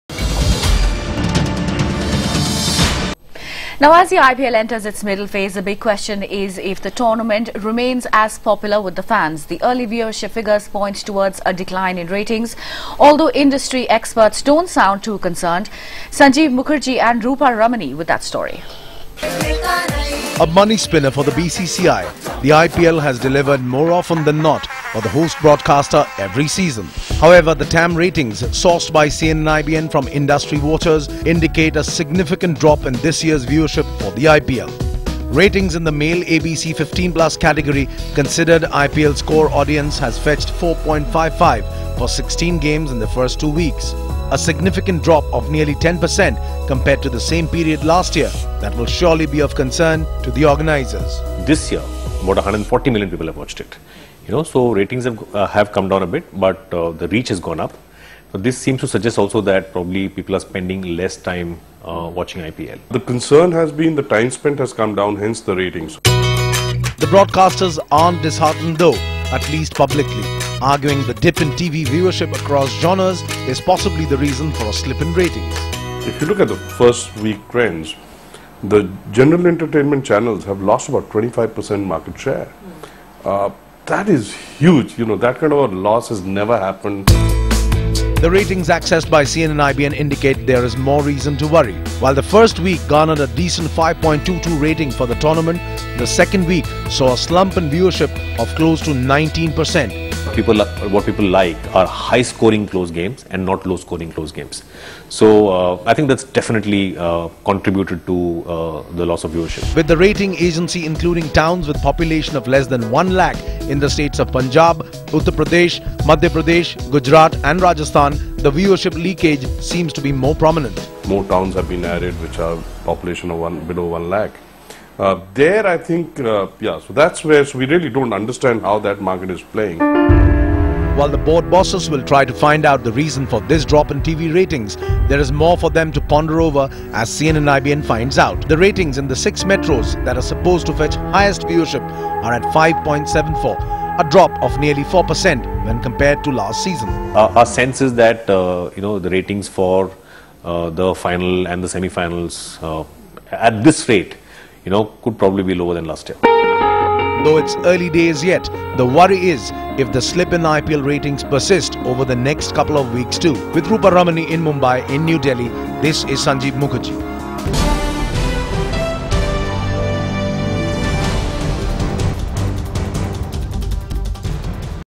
IPL 6 ratings on the decline
As the IPL enters its middle phase, the big question is whether the tournament remains as popular with the fans? The early viewership figures point towards a decline in ratings, although, industry experts don't sound too concerned. Sanjeeb Mukherjea and Rupha Ramani have the story.